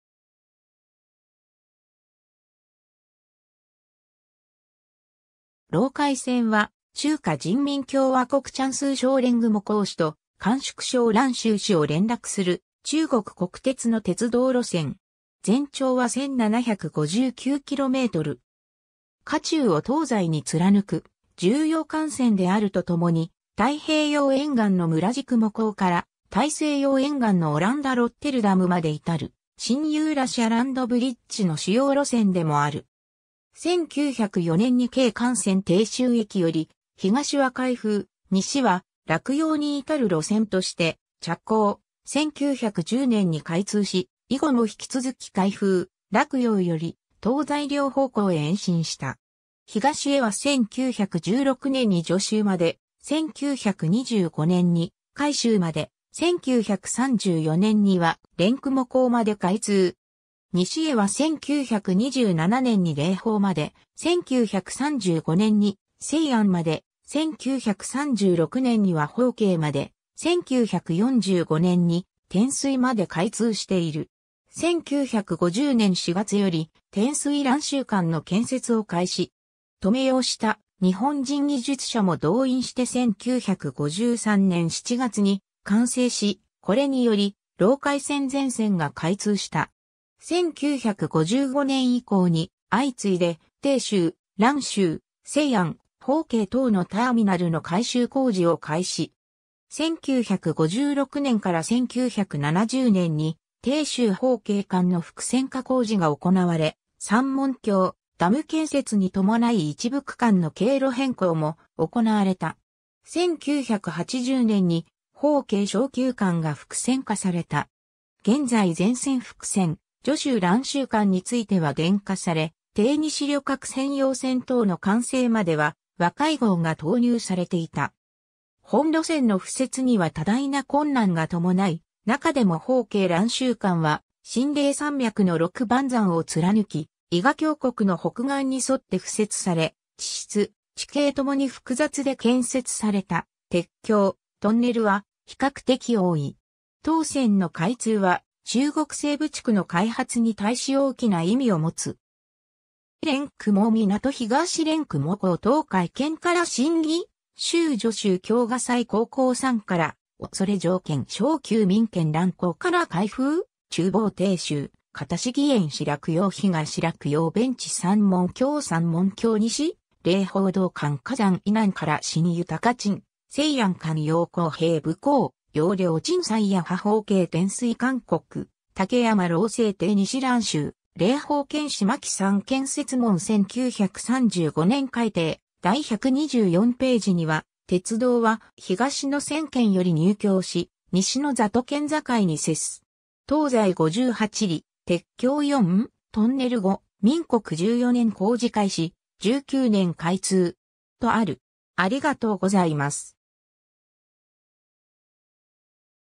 隴海線
隴海線（ろうかいせん）は、中華人民共和国江蘇省連雲港市と甘粛省蘭州市を連絡する中国国鉄の鉄道路線。
全長は1,759km。
華中を東西に貫く重要幹線であると共に、太平洋沿岸の連雲港から大西洋沿岸のオランダロッテルダムまで至る新ユーラシア・ランドブリッジの主要路線でもある。
 1904年（光緒30年）に京漢線鄭州駅より、東は開封（汴）、西は洛陽に至る路線（汴洛鉄路）として着工、1910年（宣統2年）に開通し、以後も引き続き開封、洛陽より東西両方向へ延伸した。
東へは1916年（民国5年）に徐州まで、1925年（民国14年）に海州まで、1934年（民国23年）には連雲港まで開通。
西へは1927年（民国16年）に霊宝まで、1935年（民国24年）に西安まで、1936年には宝鶏まで、1945年（民国34年）に天水まで開通している。
1950年4月より天水～蘭州間（天水線）の建設を開始。
留用した日本人技術者も動員して1953年7月に完成し、これにより隴海線全線が開通した。
 1955年以降に相次いで鄭州、蘭州、西安、宝鶏等のターミナルの改修工事を開始。
1956年～1970年に鄭州～宝鶏間の複線化工事が行なわれ、三門峡ダム建設に伴い一部区間の経路変更も行なわれた。
1980年に宝鶏～商丘間が複線化された。
現在全線複線、徐州～蘭州間については電化され、鄭西旅客専用線等の完成までは和諧号が投入されていた。
 本路線の敷設には多大な困難が伴い、なかでも宝鶏～蘭州間は秦嶺山脈の六盤山を貫き、渭河峡谷の北岸にそって敷設され、地質、地形共に複雑で建設された鉄橋、トンネルは比較的多い。
当線の開通は中国西部地区の開発に対し大きな意味を持つ。